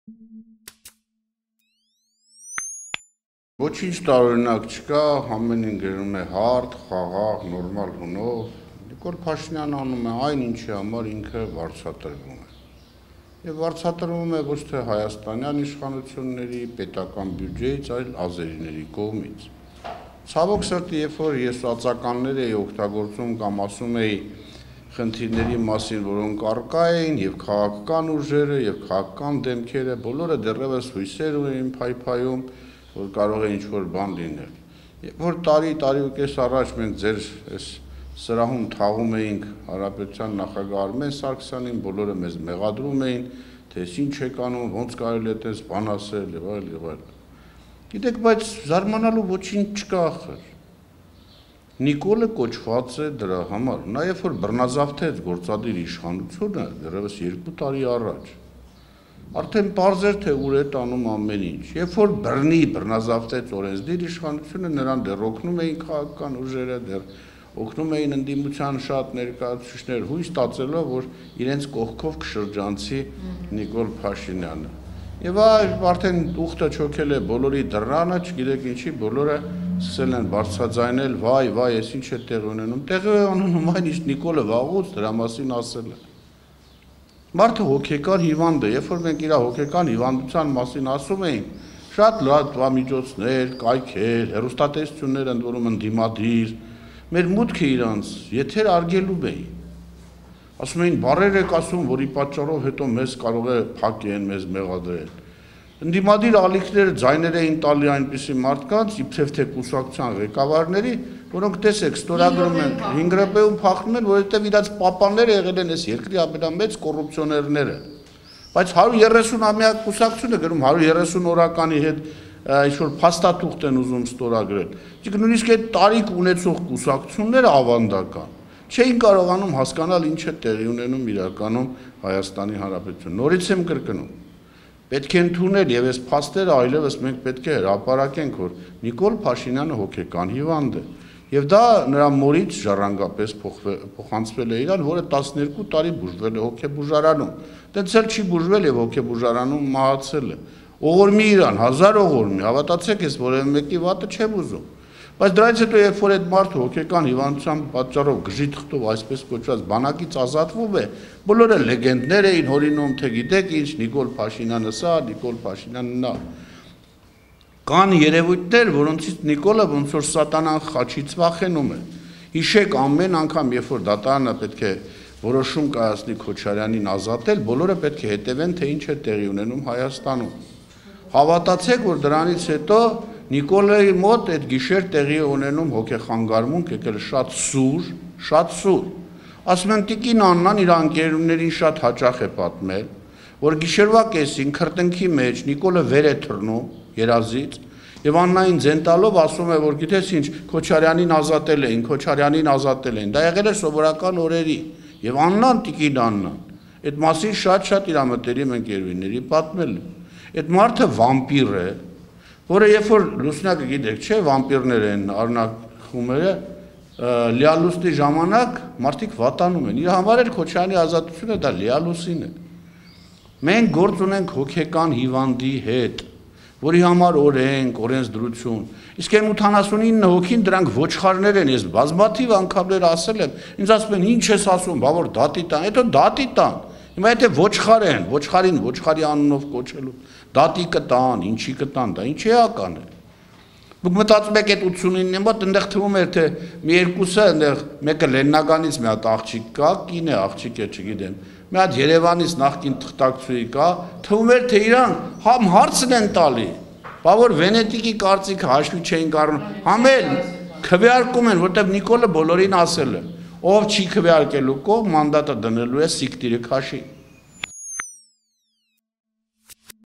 Նիկոլ Փաշինյանը հոգեկան հիվանդ է. Գառնիկ Իսագուլյան
Արնախումերը լիալուսնի ժամանակ վատանում են, Փաշինյանի համար էլ Քոչարյանի ազատությունը լիալուսին է. կարծիք